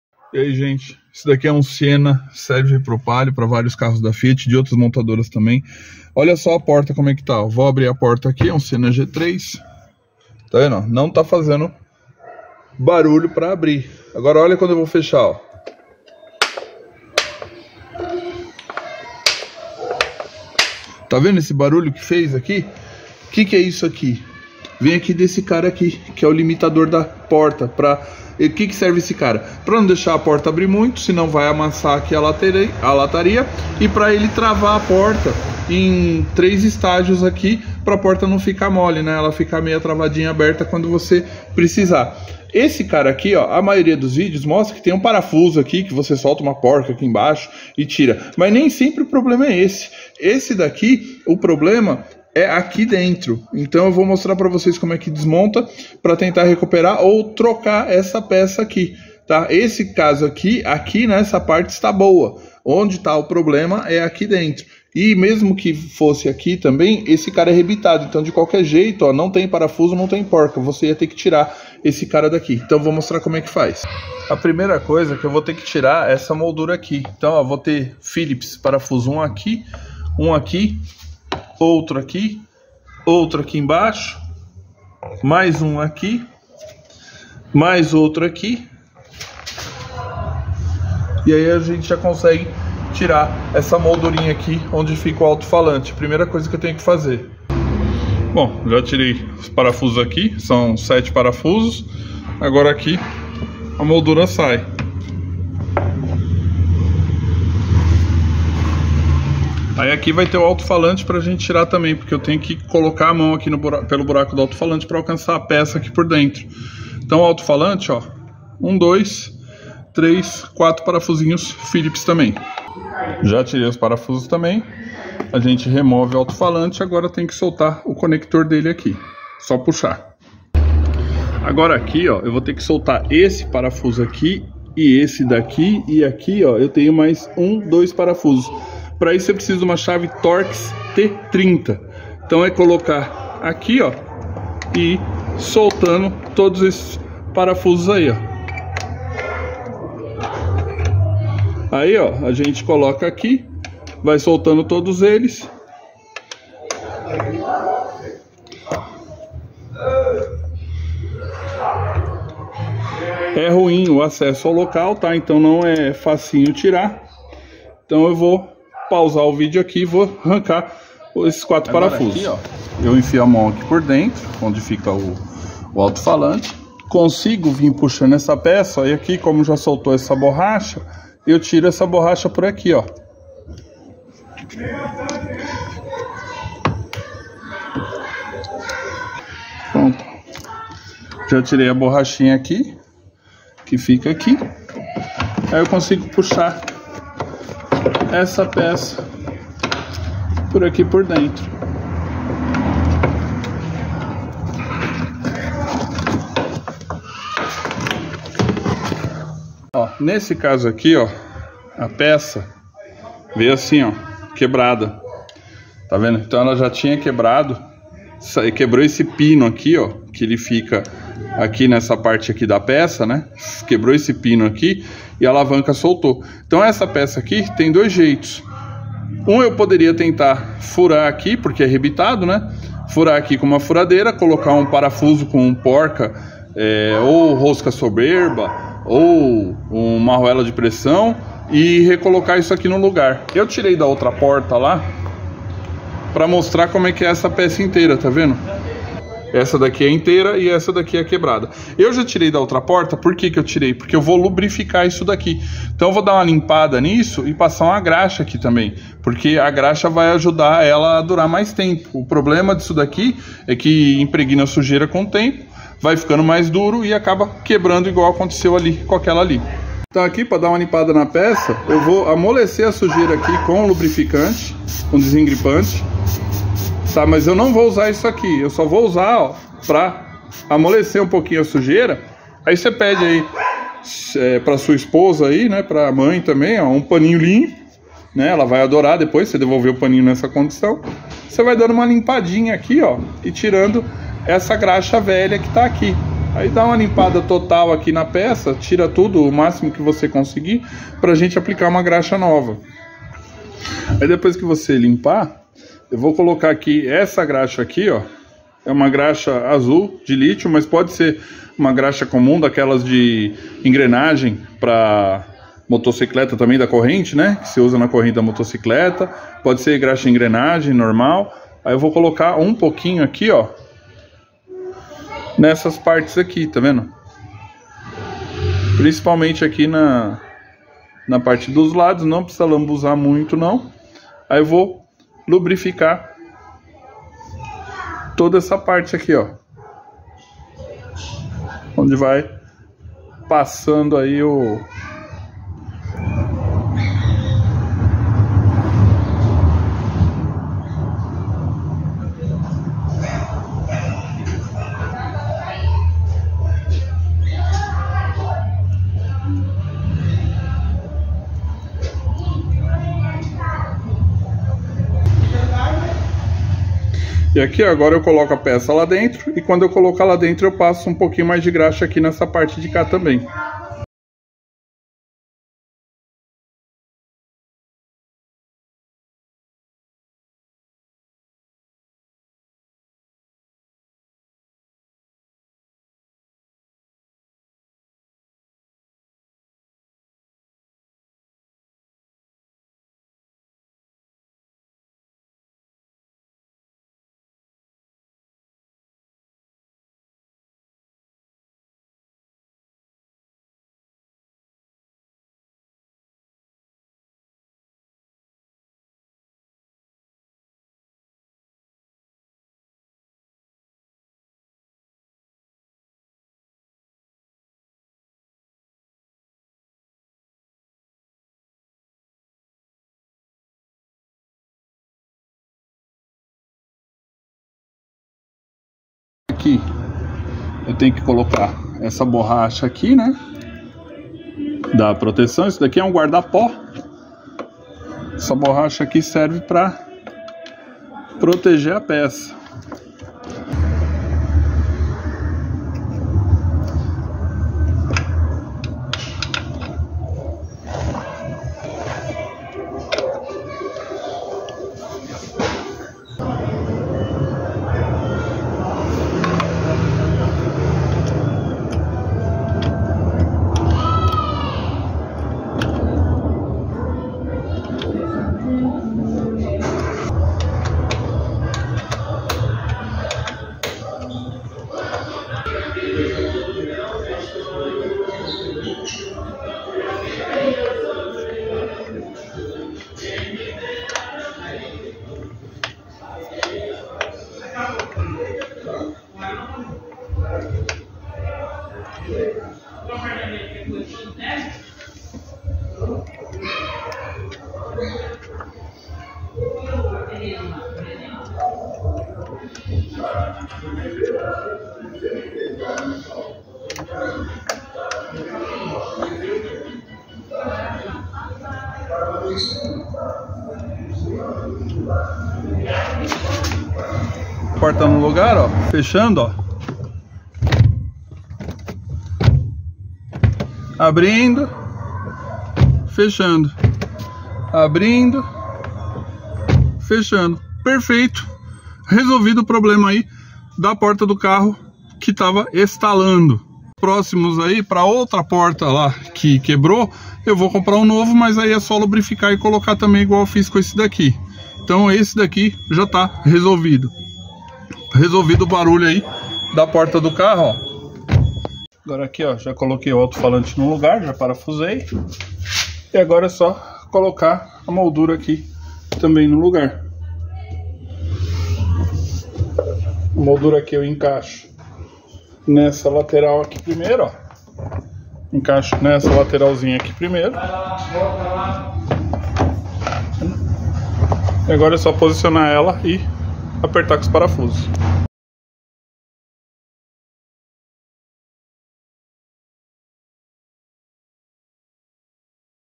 Porta dianteira Fiat Palio Siena Estalando - Limitador da porta troca desmontagem e lubrificação
Porta Fiat Palio Siena G3 Estalando Estralando com Barulho Rangendo
Portas da frente - Troca e lubrificação do limitador de porta nhec nhec
Desmontar forro da porta dianteira